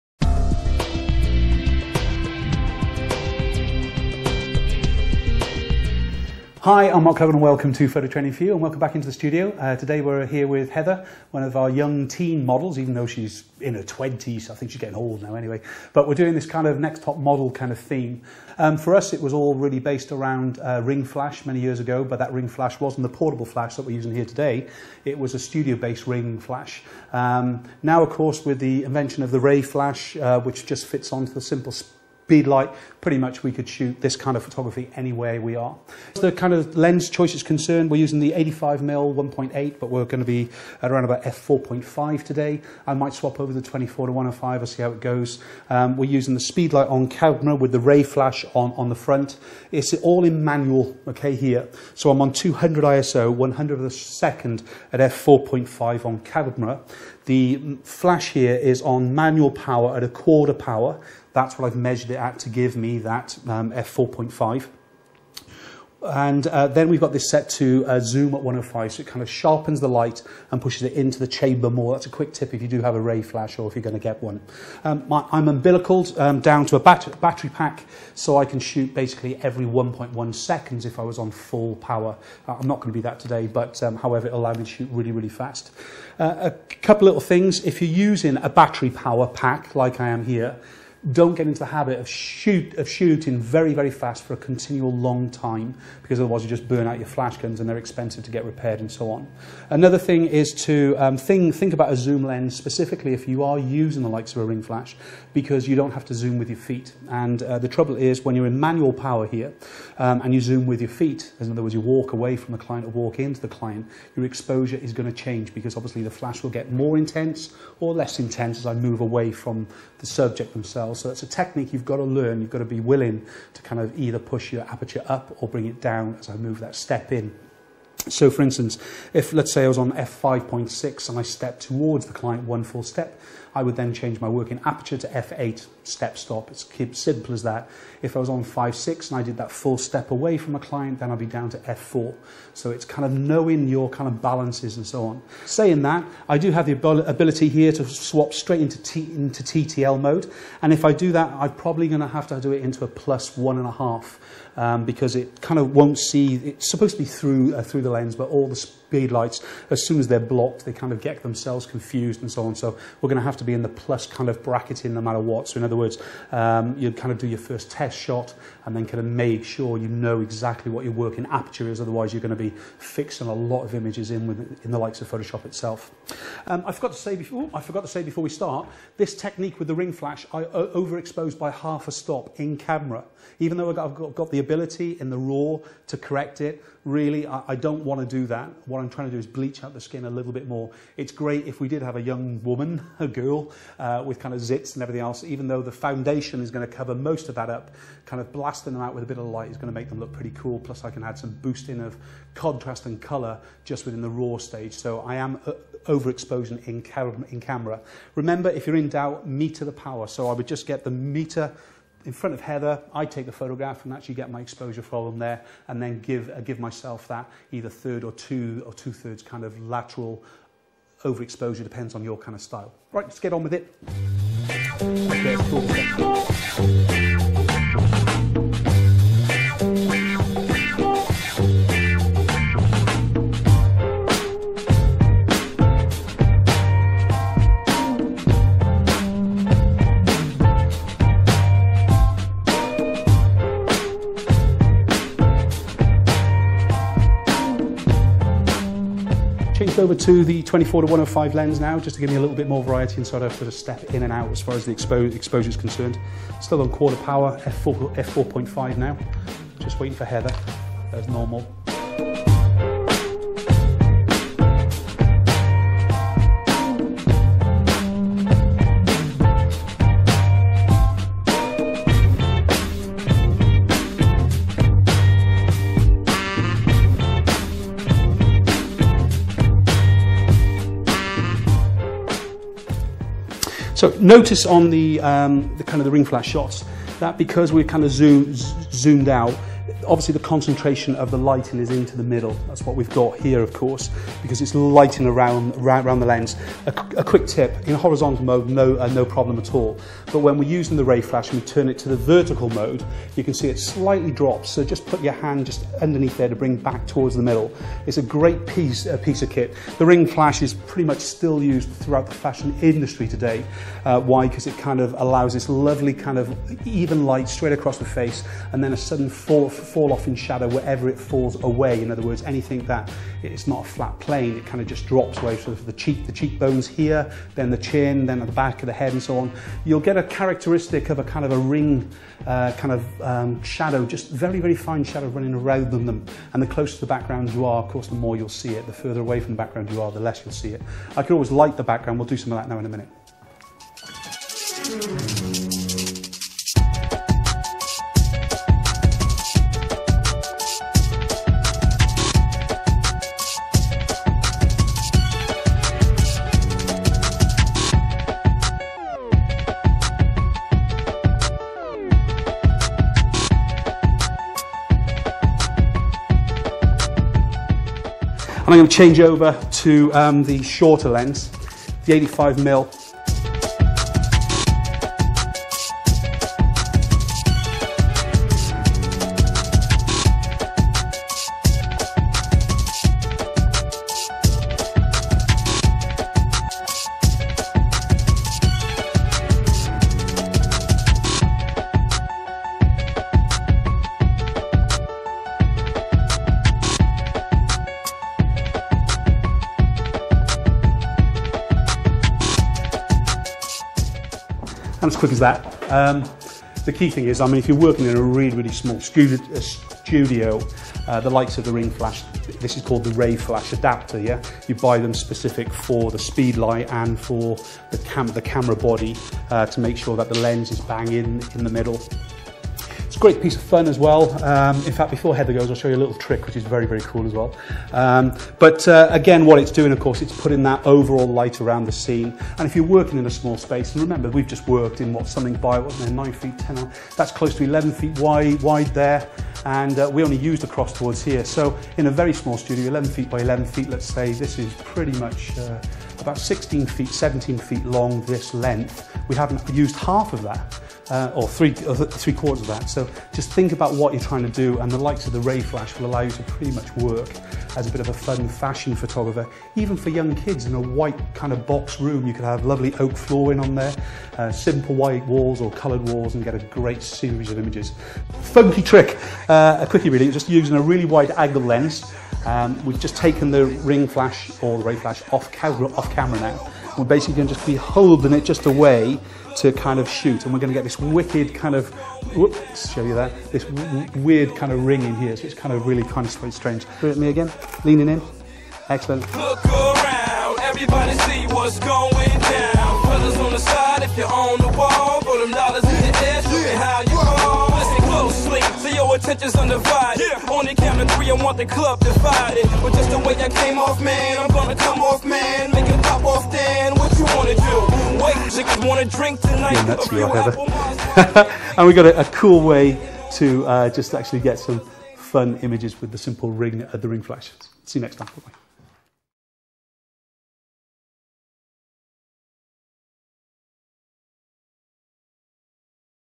Shooting Next Top Model with Ringflash
At PT4U we are huge fans of the RayFlash Ringflash Adapter. We made this hort video to give you an insight on how we use it to shoot our very successful Next Top Model promotions.